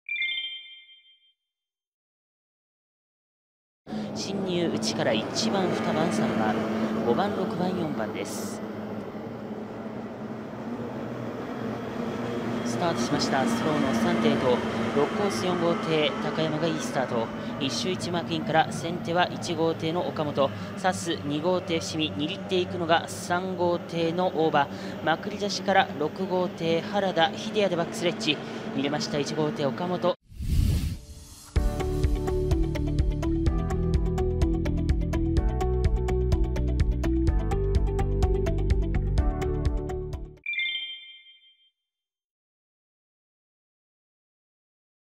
BOATCAST NEWS│岡本慎治 大記録！通算2600勝に リーチ!! ボートレースニュース 2022年8月3日│
岡本慎治選手、通算2600勝に リーチ!!
浜名湖一般 最終日6Rをご覧ください！